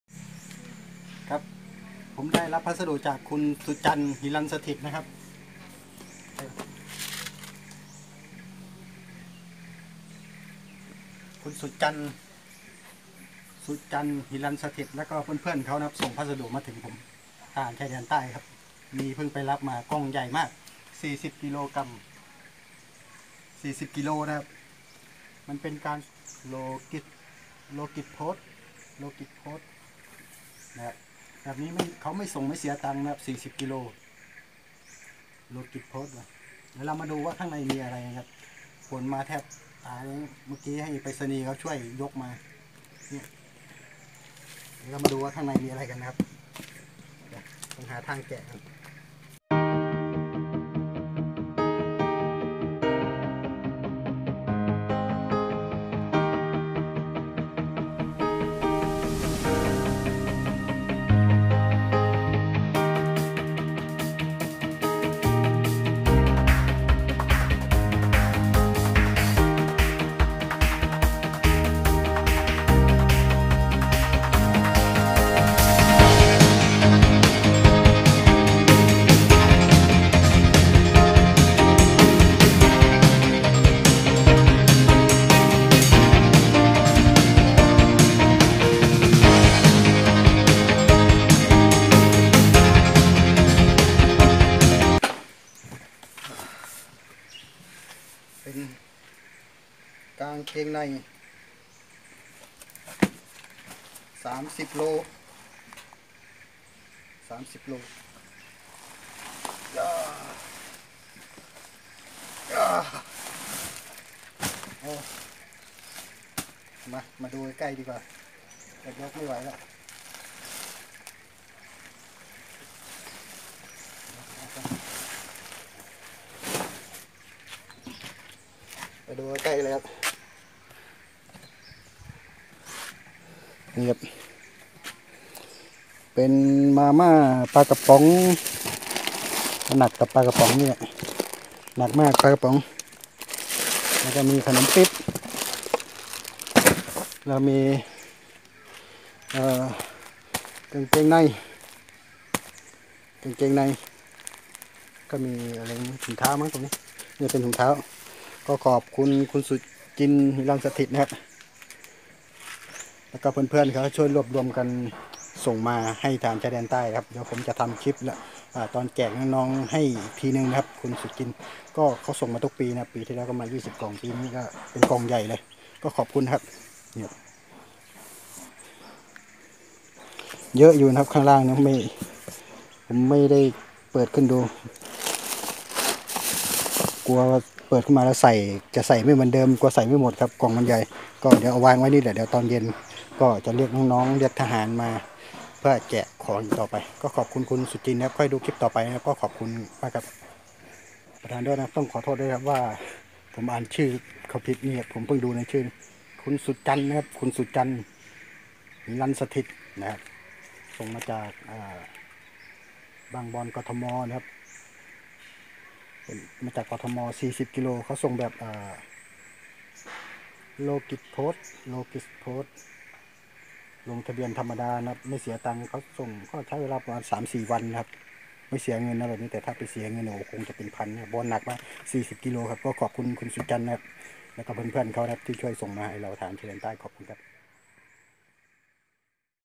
ขอบคุณพัสดุจากแฟนเพจลูกพระเจ้าตาก คุณสุจันทร์ และเพื่อนๆครับ
ขอบคุณกำลังใจและพัสดุจากแนวหลังที่ส่งถึงเราทหารชายแดนใต้นะครับ